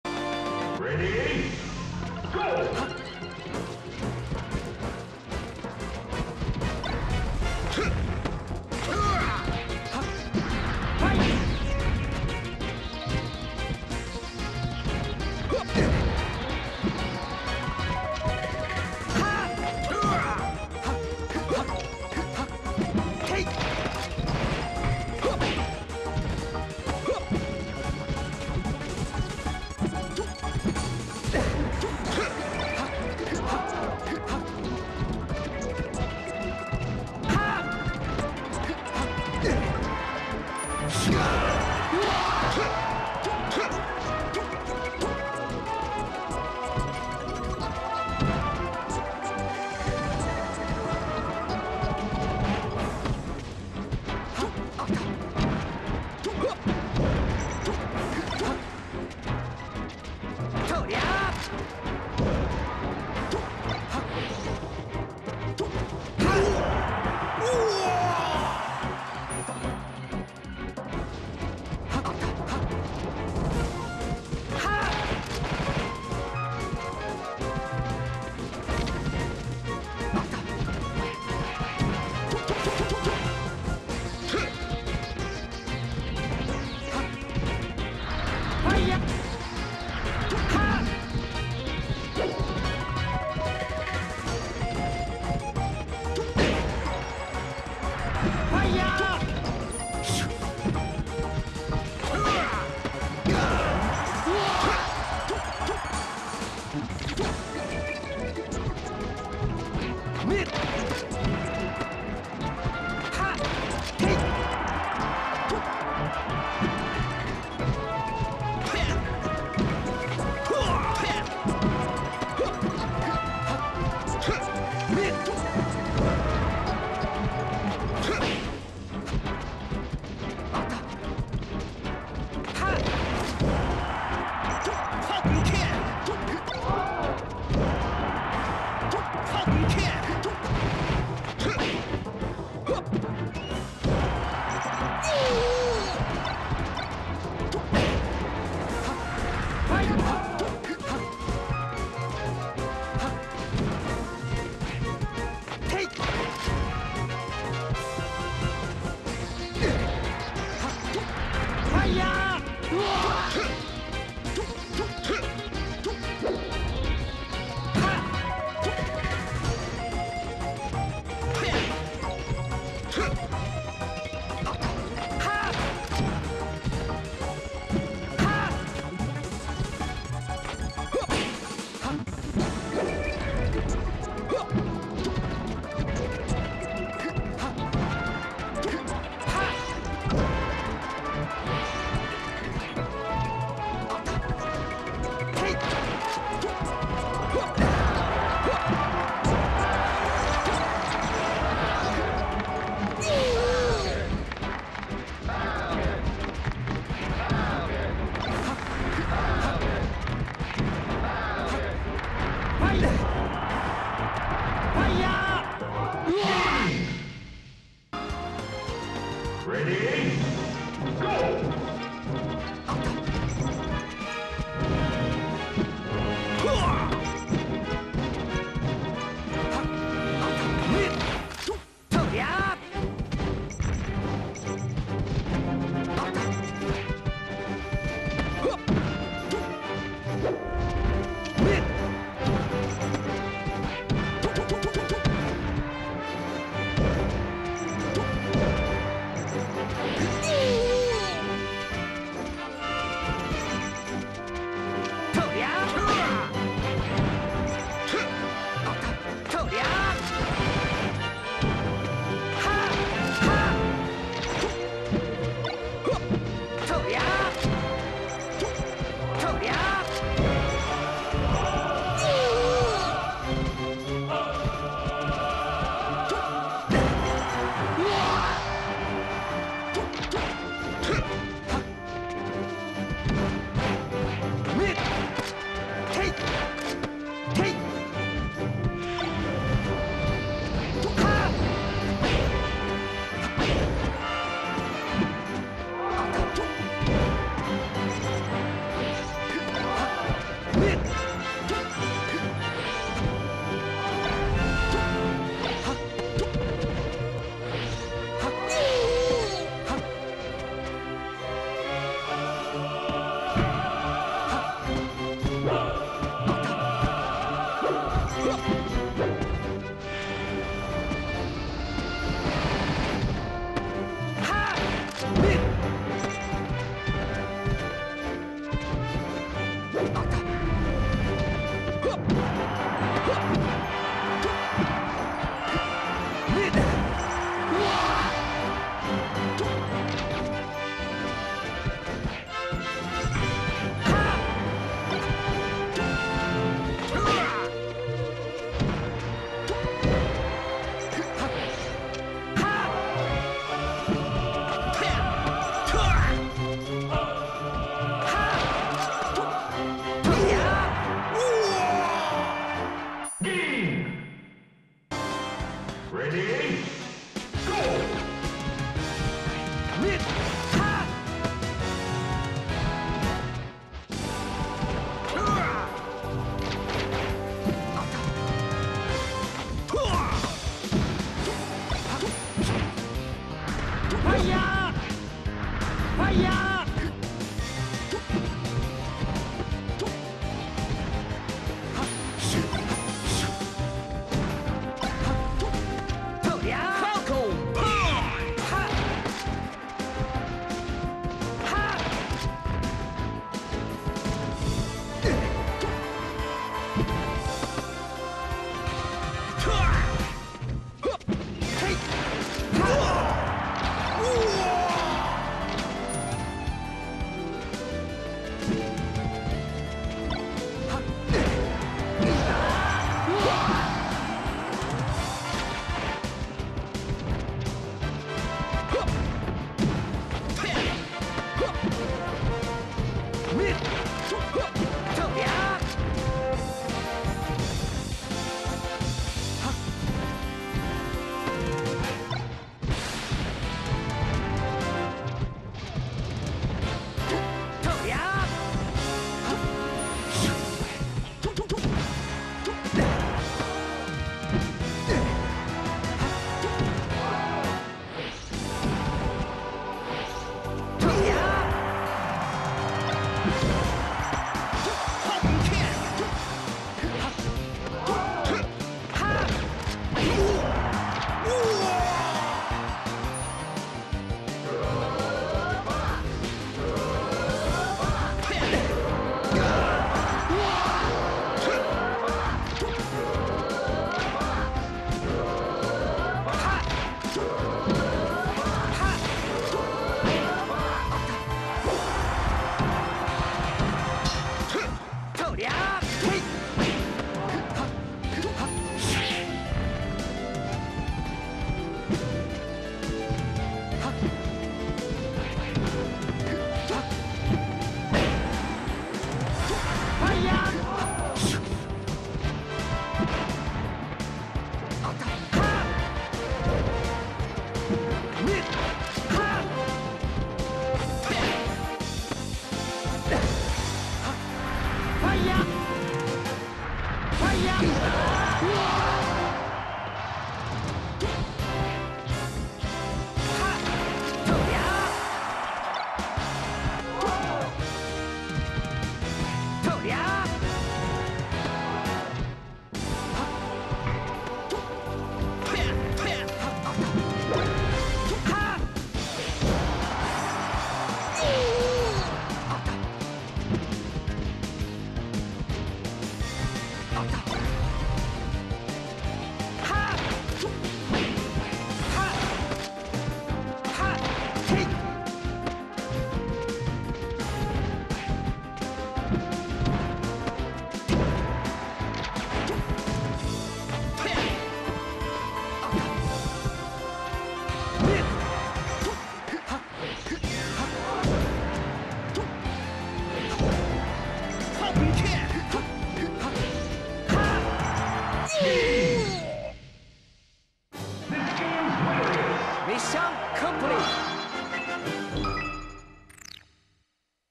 Apex 2012: Mafia (Falcon) vs Codi (Fox) MM
Apex 2012
Day 1: January 6th
New Brunswick, NJ
Money Match